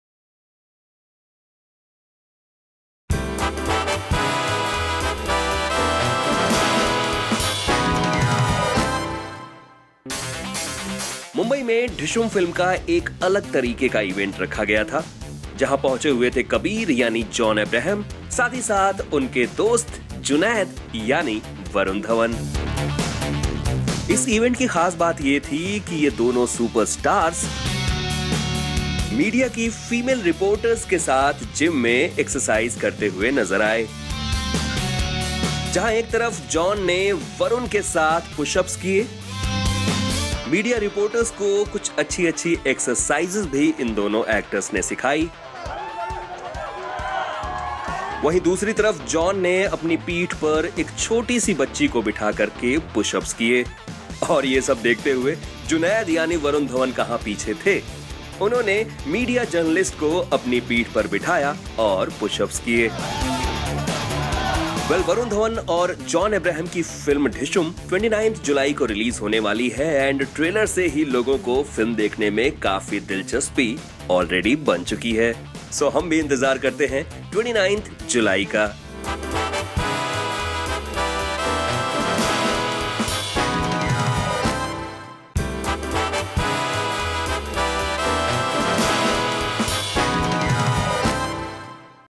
Varun Dhawan & John Abraham ‘Workout’ with the Fans | Bollywood News | ErosNow eBuzz
Dishoom Stars Varun Dhawan & John Abraham enthrall fans with their workout moves.